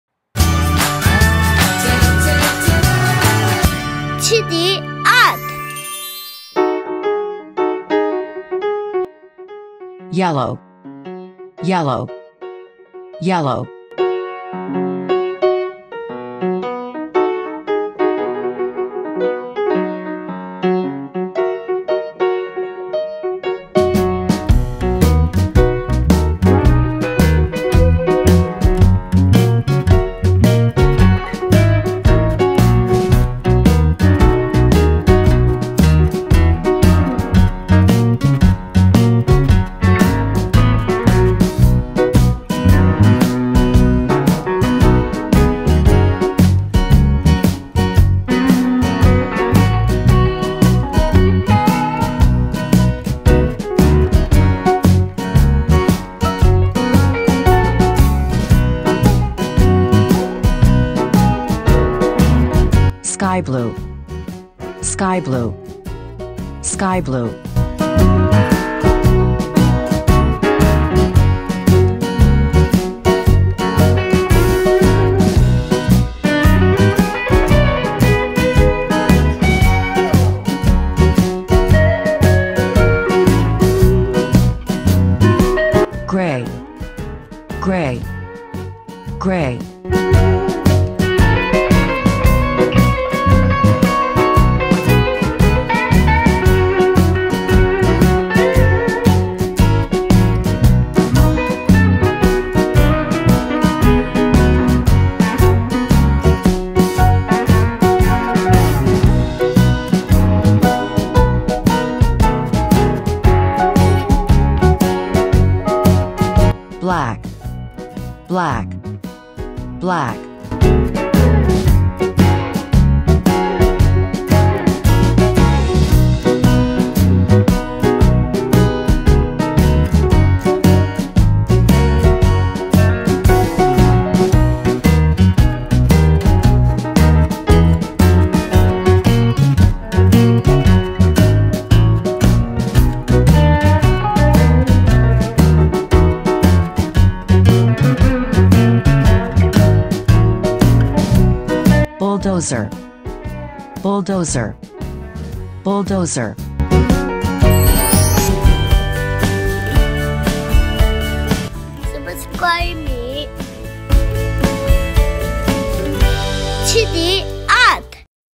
Bull dozer coloring | Coloring Pages for BULL DOZER for kids | Learning ALPHABET
Hi~ Kids!
I am Jeenie~!
Today, I am going to draw BULL DOZER!
BULL DOZER is the one of the strongest heavy vehicle you may like it!